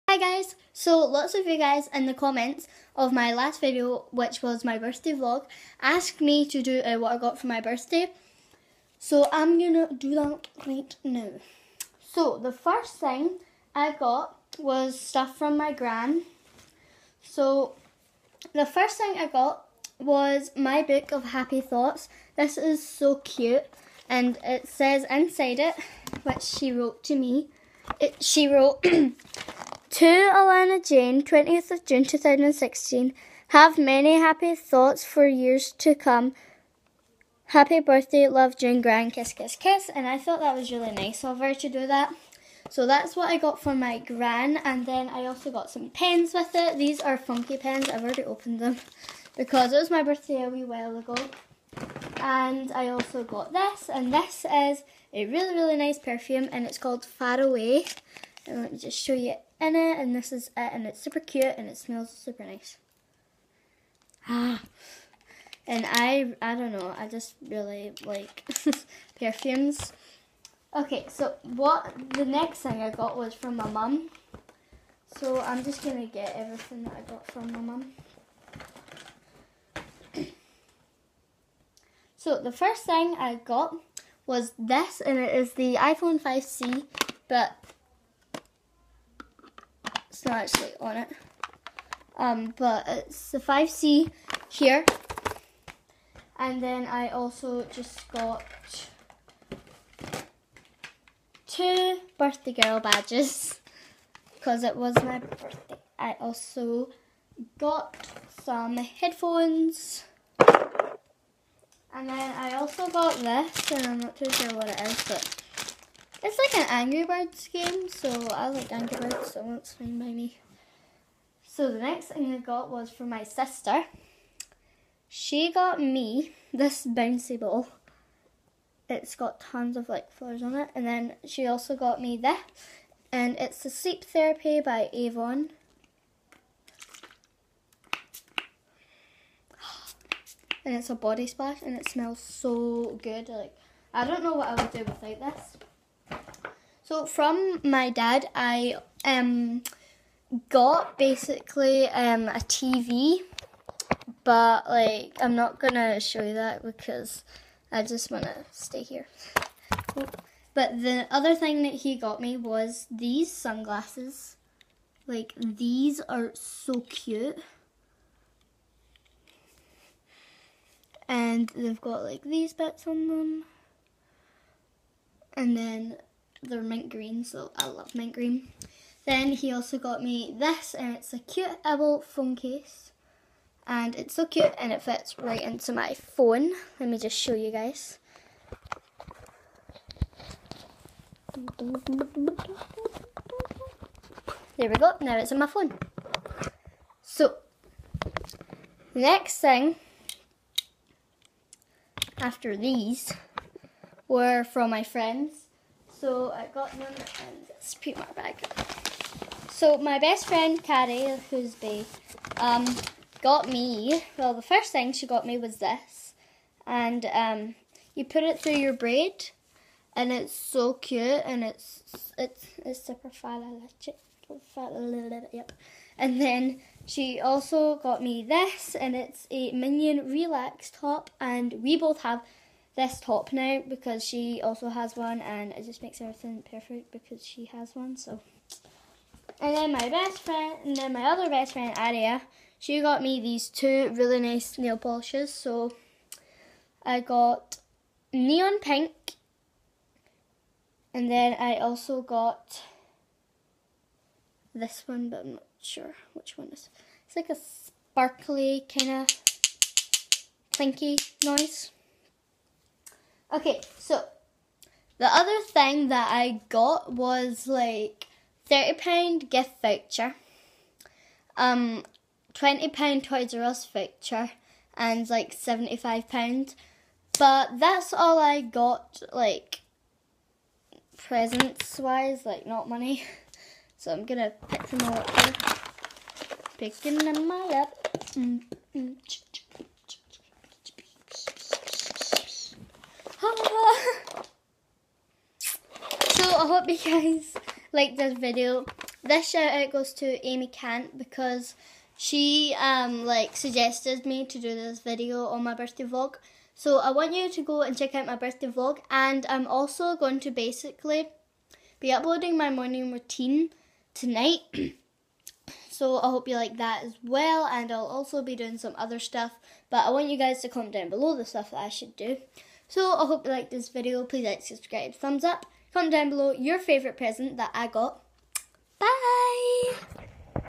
WHAT I GPT FOR MY BIRTHDAY 🌙☄( 2016 )
Hi it was my bDay 3 days ago so I'm doing a what I got got my birthday

This was commented by lots of people
I hope your enjoy

Shoutout-Amy Cant

MUSIC.LY-queen_alannajane

Byeeeeeeee